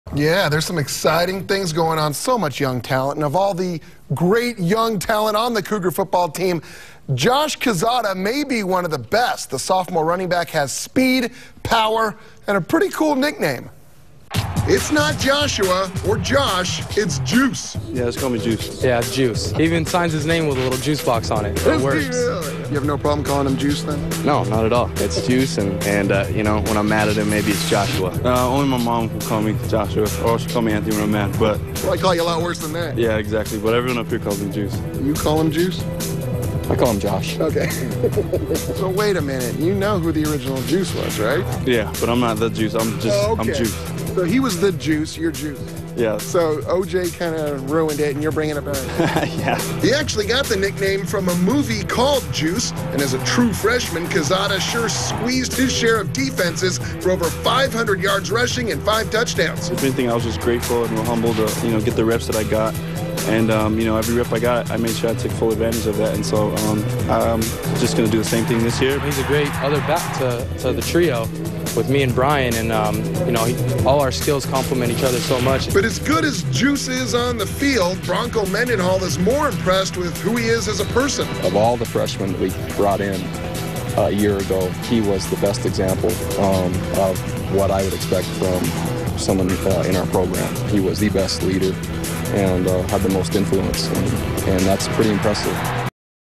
Josh Juice Quezada
Josh Juice Quezada  Pre Season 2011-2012 Sophomore Year BYU Running Back